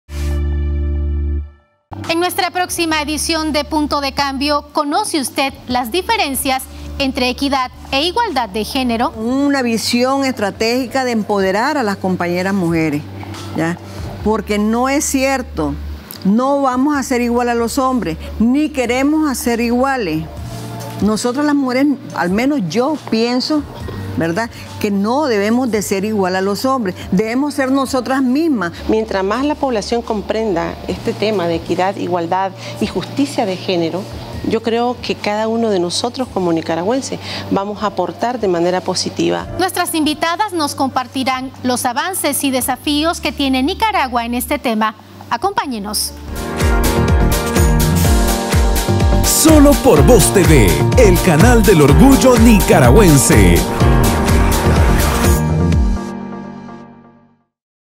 PUNTO DE CAMBIO: Diferencia entre igualdad y equidad de género
Mientras la población comprenda estos temas de igualdad, equidad y justicia de género, cada uno de nosotros como nicaragüenses vamos a aportar más a esta temática.